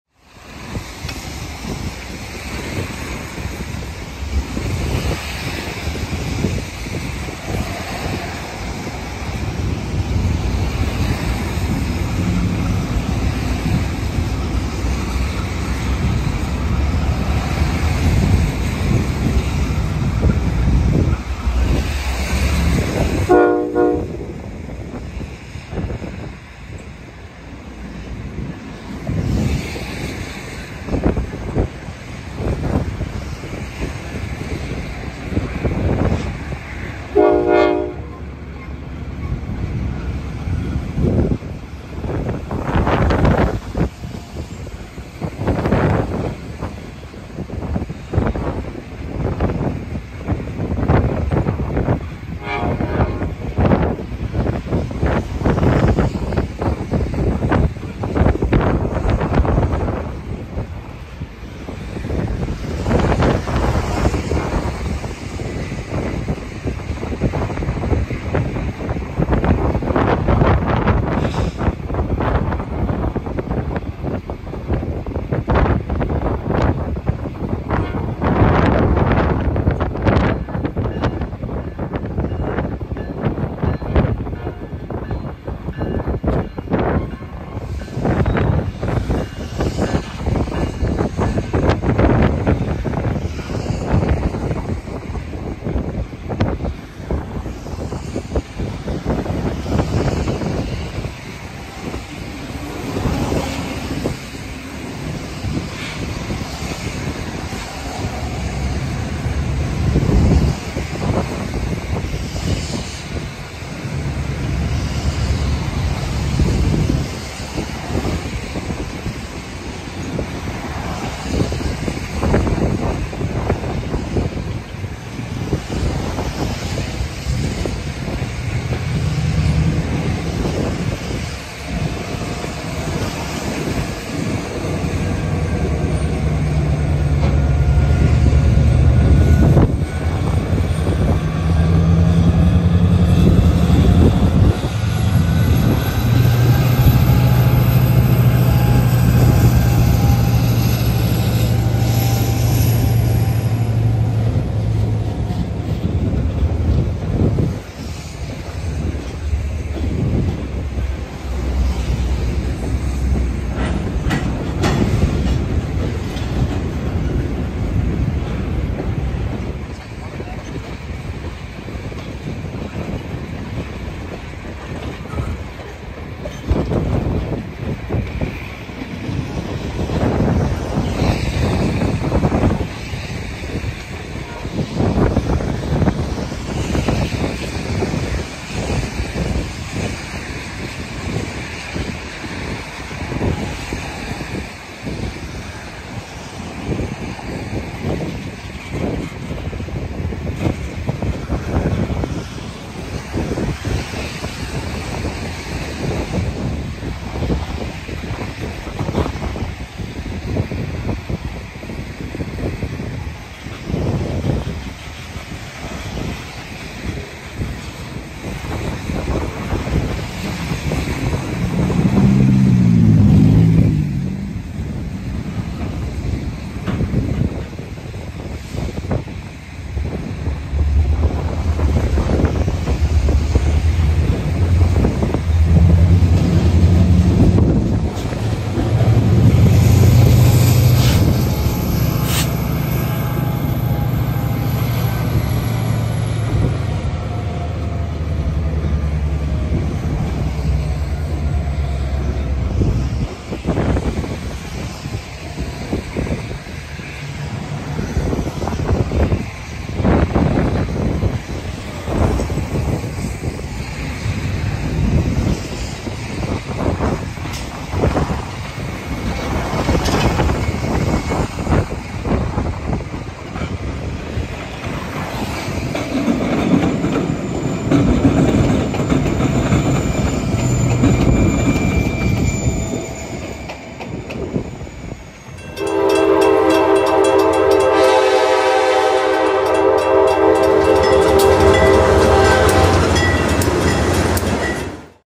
Railfanning the Belt Railway of Chicago
On a cold and windy afternoon we are looking over the belt railway of Chicago's clearing yard. We would catch a few moves before getting back in the warm.

Main channel: @MichiganCentralLines